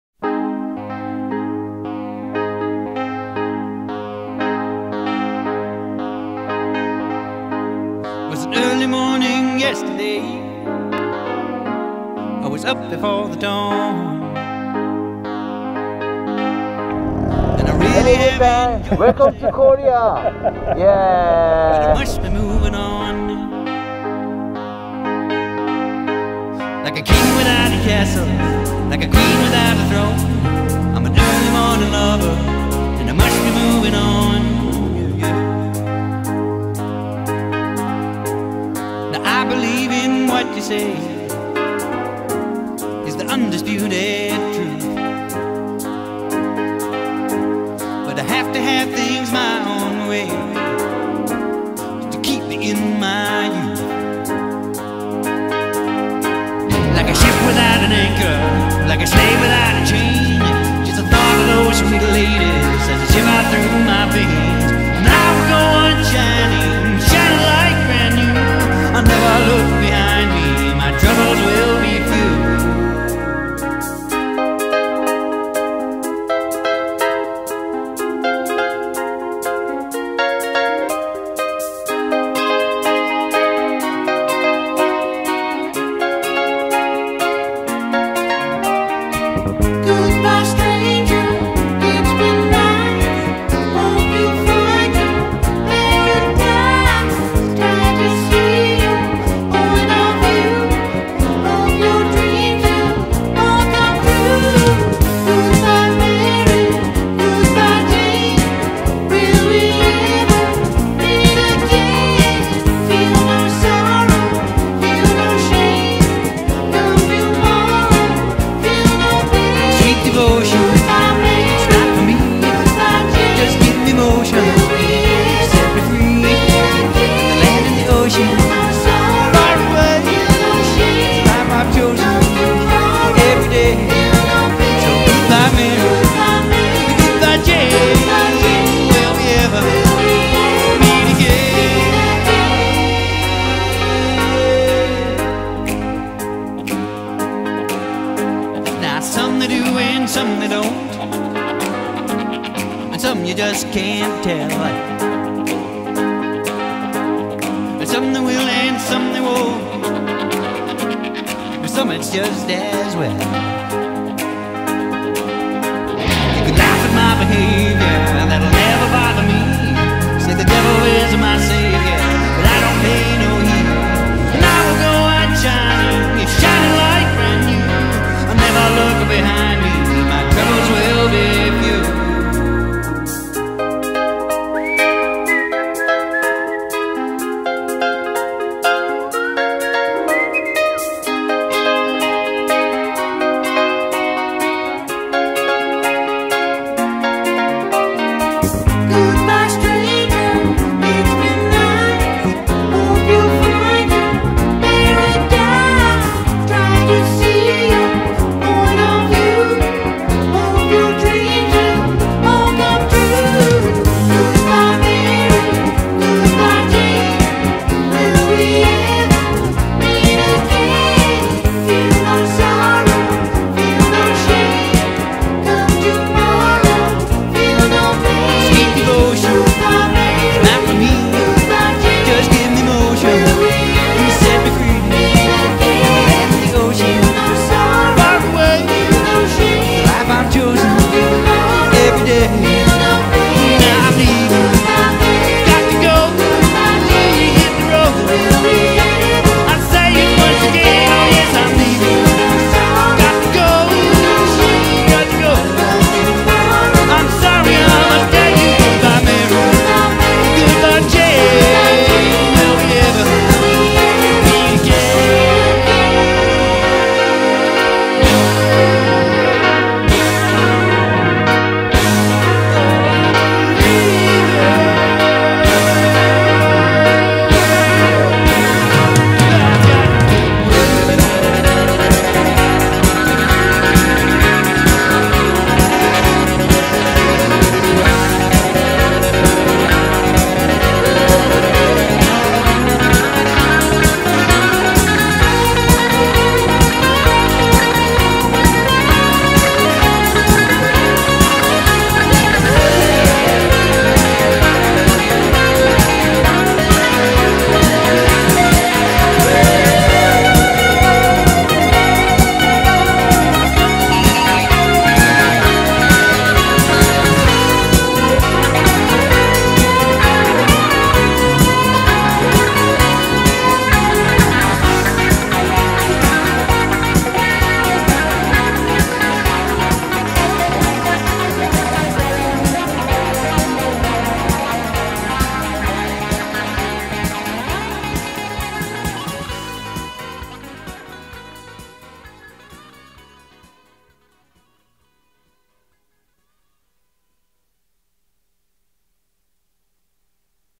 YAMAHA XTZ 750 Super Ténéré - "THE ROAD" AROUND THE WORLD ON A 1991 SUPER TENERE
YAMAHA XTZ 750 Super Ténéré - AROUND THE WORLD - (SCOTLAND, NORTH AMERICA, CANADA, SOUTH KOREA, RUSSIA, EUROPE, ENGLAND, SCOTLAND) 2018